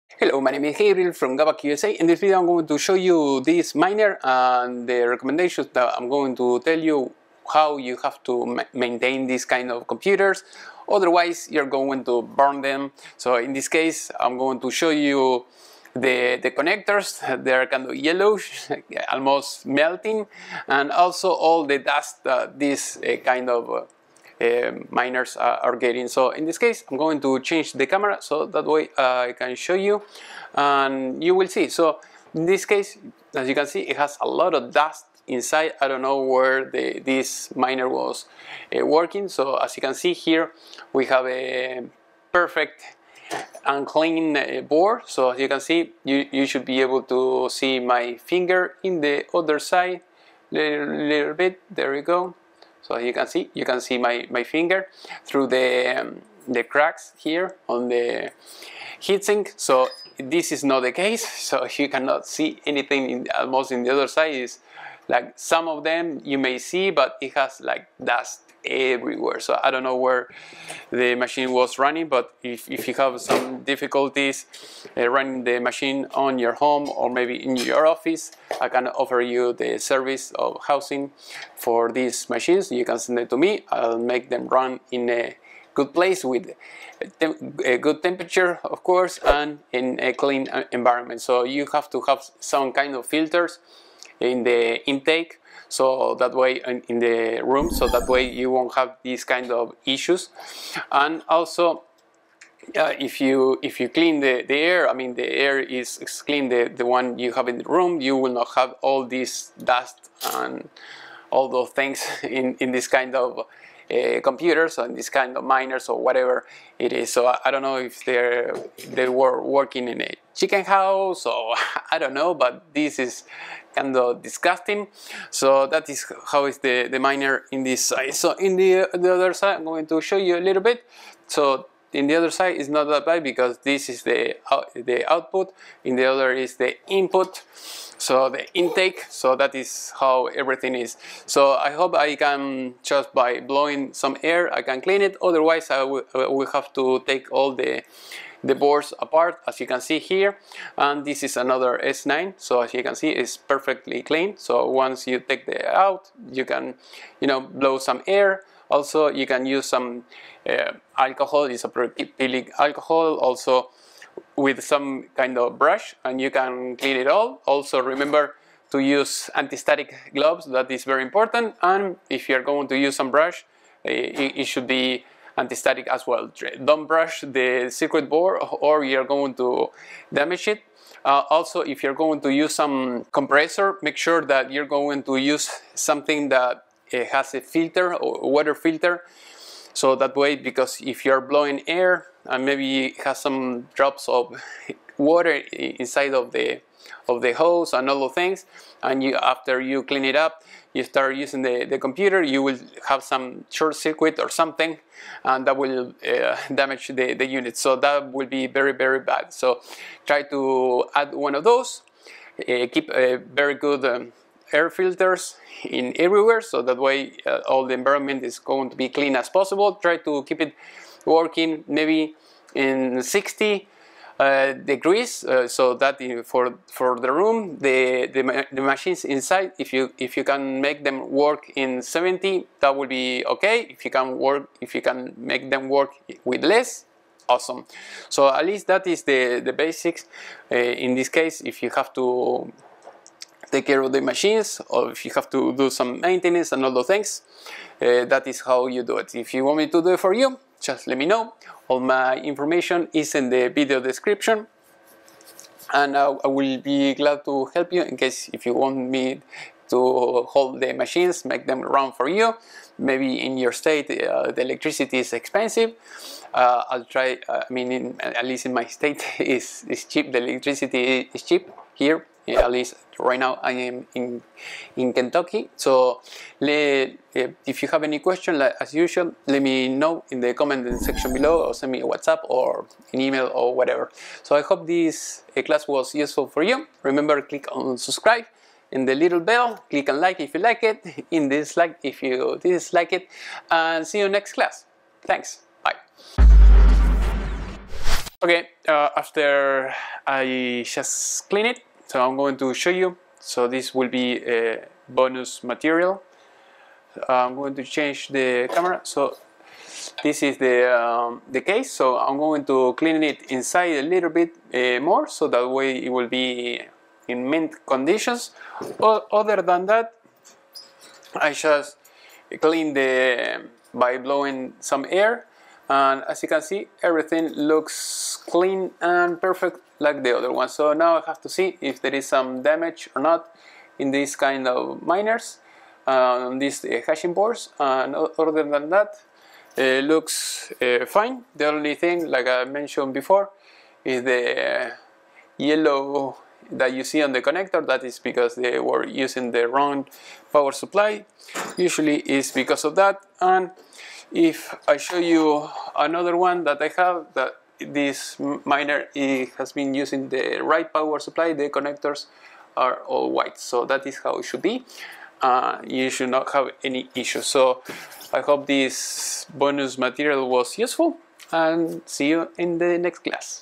maintenance & temperature of miners antminer
starting a bitcoin miner farm? here I show you all you need to know to know about how to keep them clean and guide to make them run for long time

QUESTIONS?

PERISCOPE: GabakUSA